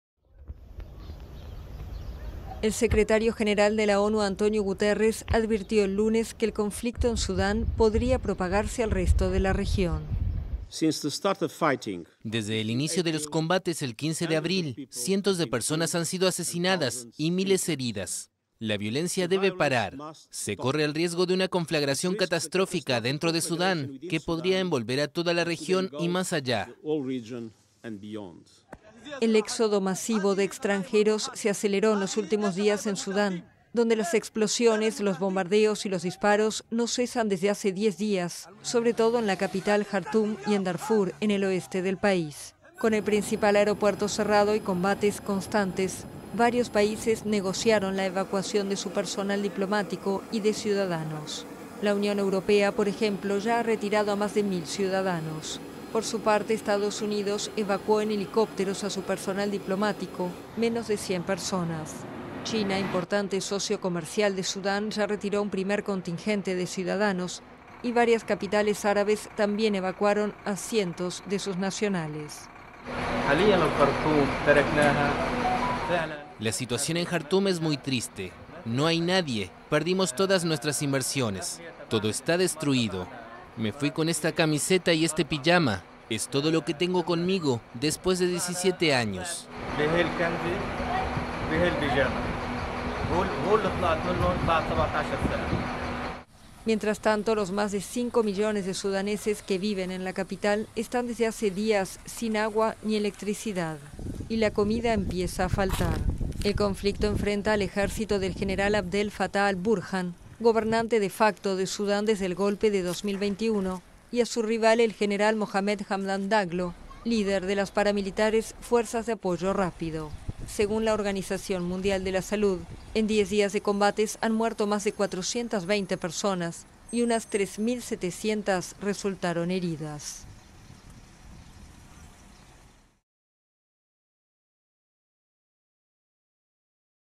Conflicto en Sudán amenaza con propagarse a toda la región, dice jefe de ONU | AFP
El secretario general de la ONU, Antonio Guterres, instó el lunes a que pare la violencia en Sudán ya que el conflicto amenaza con propagarse por toda la región.